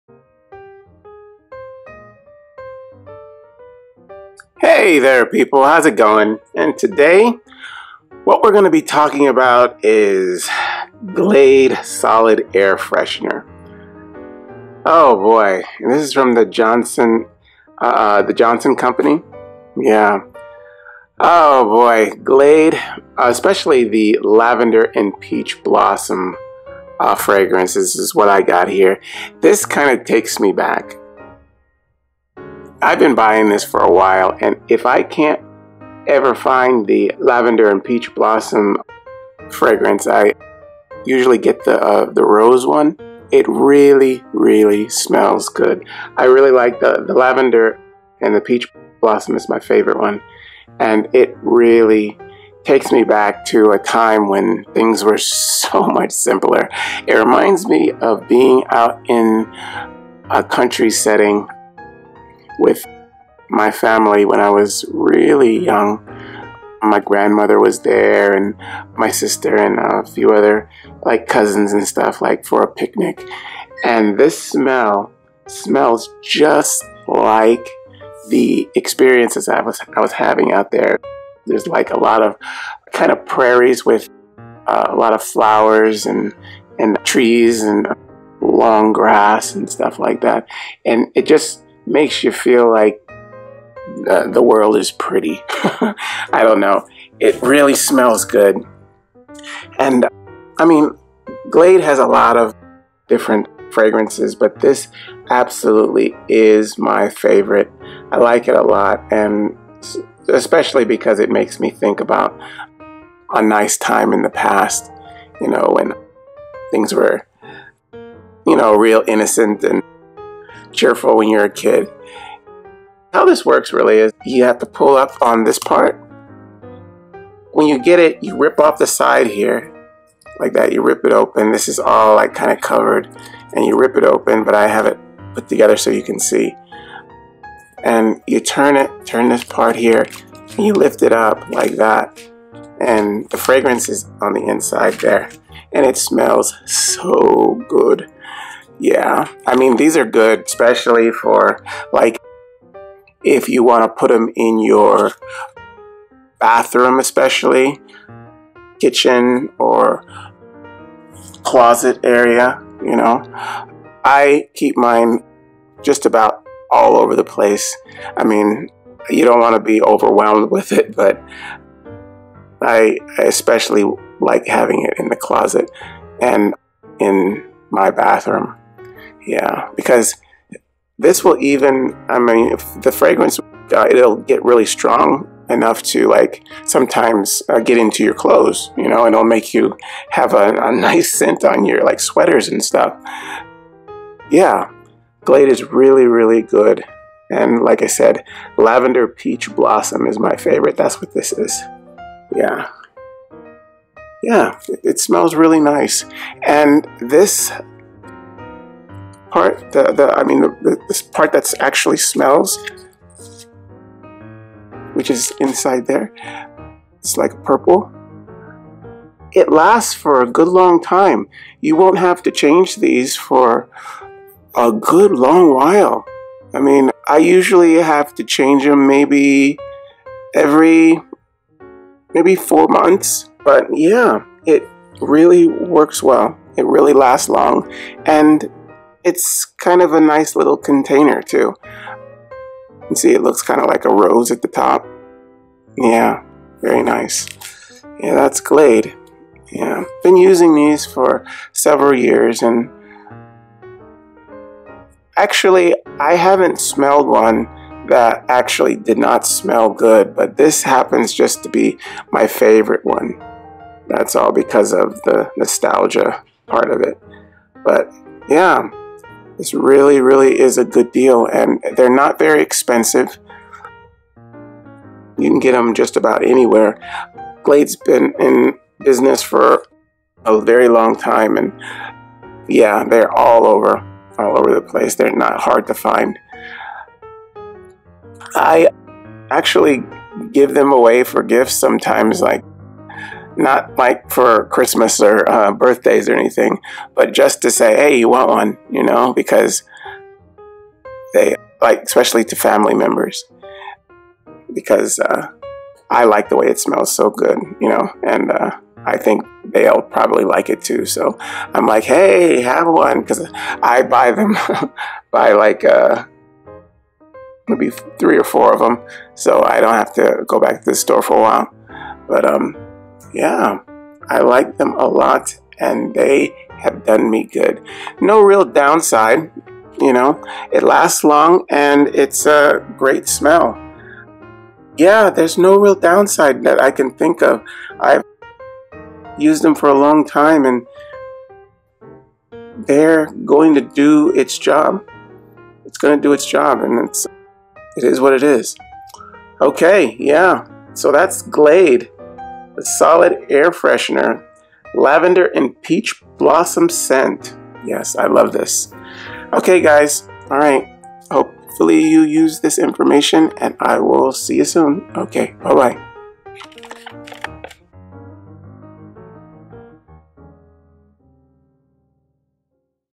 ✅ How To Use Glade Solid Lavender and Peach Air Freshener Review
Price Check
About this product:

Glade Solid Lavender and Peach Air Freshener

Just twist to release the stimulating scent of lavender and peach and set the intensity by adjusting the lid. It's the perfectly practical way to add continuous fragrance to your home.